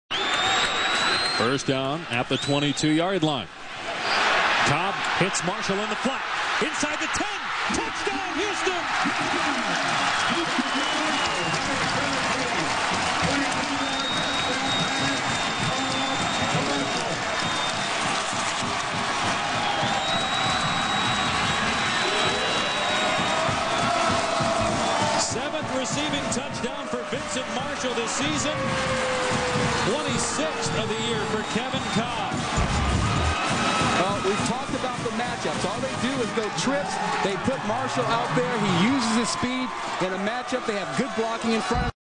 Kolb to Marshall TD Houston Cougars 2006 C-USA Champions
Kevin Kolb hits Vincent Marshall for the Coogs first TD of the championship game.